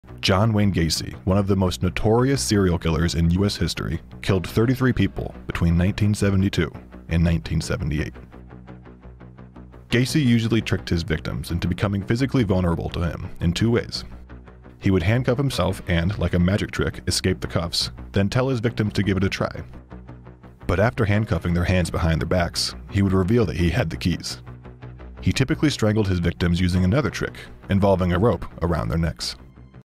Killer clown tricks his victims
John Wayne Gacy, a notorious serial killer, employed a chilling array of deceitful tactics to ensnare his unsuspecting victims. Masquerading as a friendly and charismatic figure, Gacy lured young men into his clutches through various means.

He would often present himself as a trustworthy individual, leveraging his affable personality and community involvement to gain their confidence. Offering job opportunities, money, or companionship,
Gacy exploited the vulnerability and aspirations of his victims. Once they were under his control, he would employ cunning methods such as handcuffs, drugs, or even impersonations of law enforcement to overpower them.

Gacy's ability to manipulate and deceive his victims added a disturbing layer to his heinous crimes, leaving a trail of tragedy and horror in his wake.